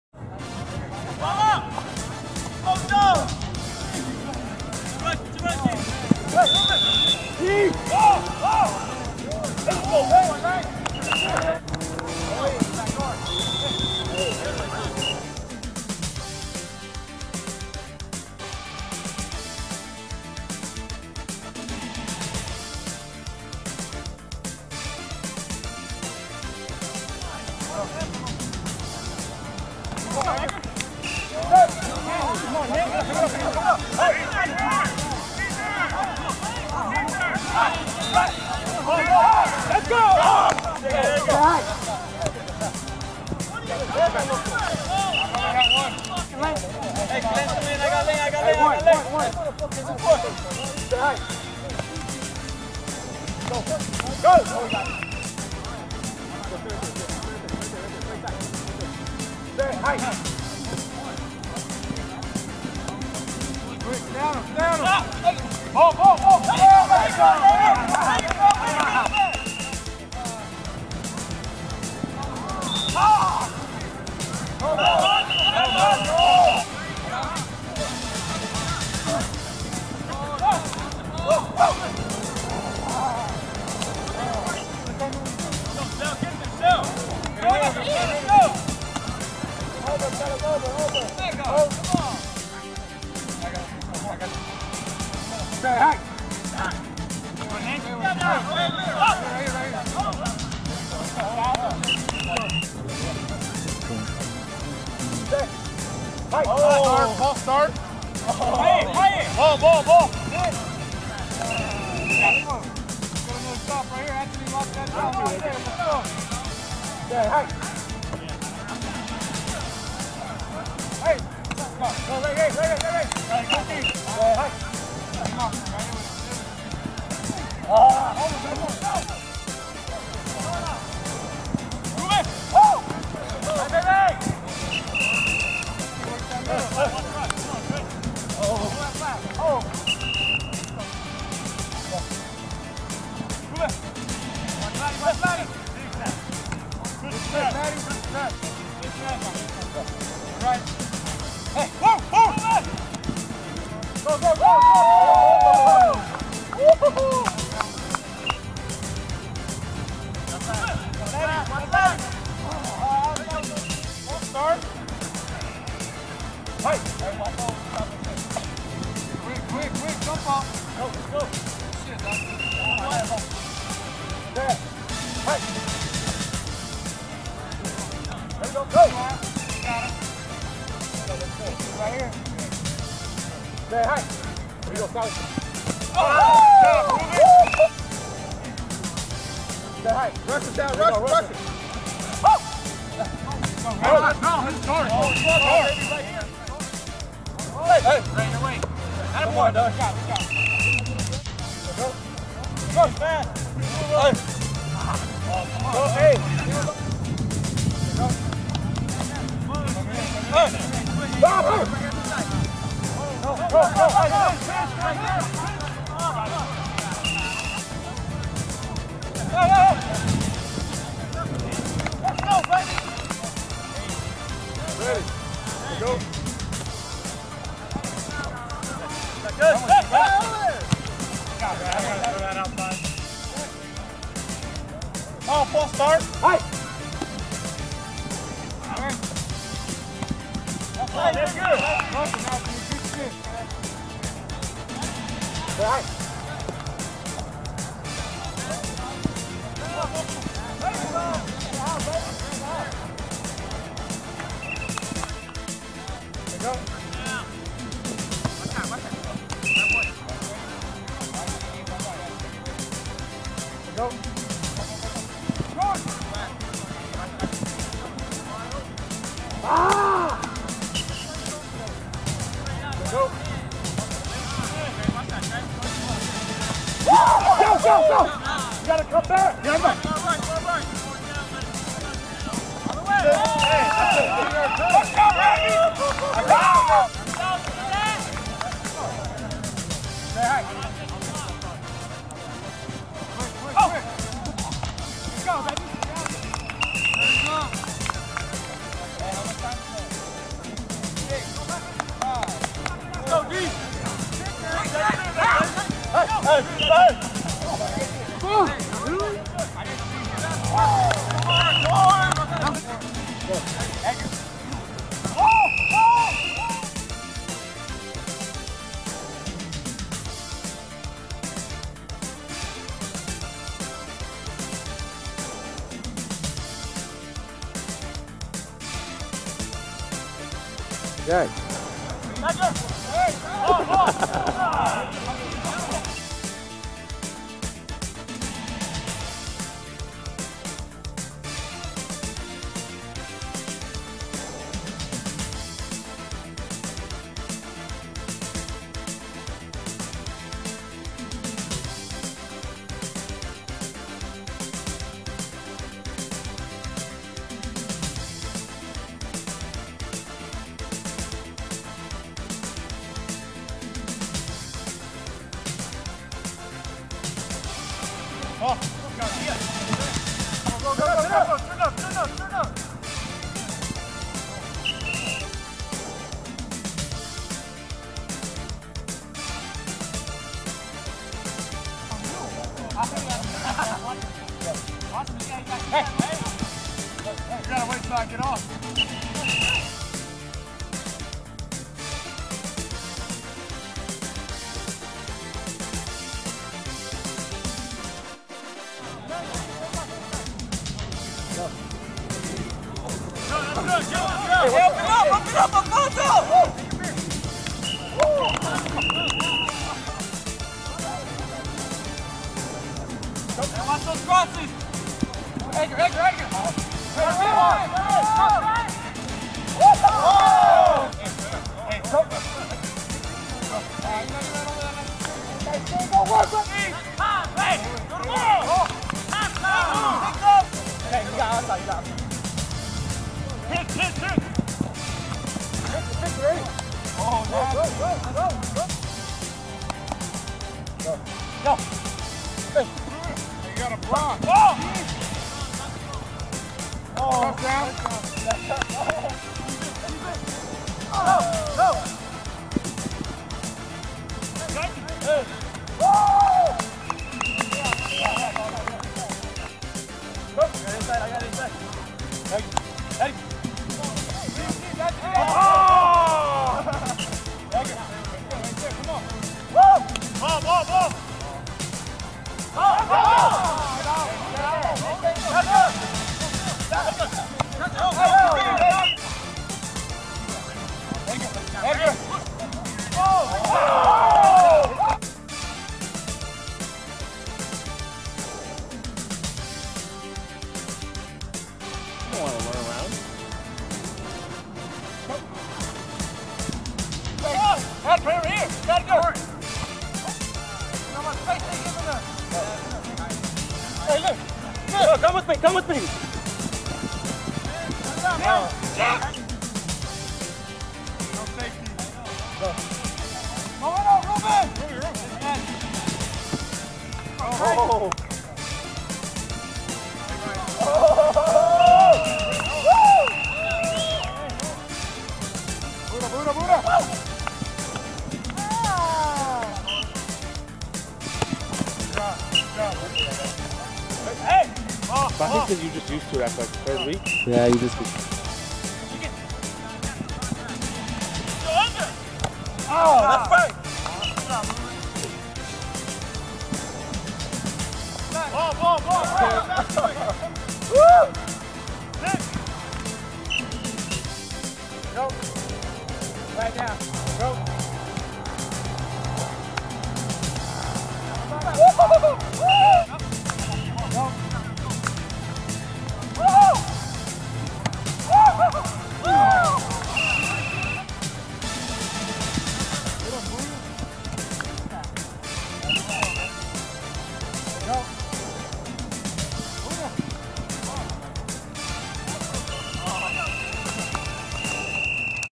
Game of the week - Punishers vs DNA
Gametime flag football - Game of the week -  Punishers vs DNA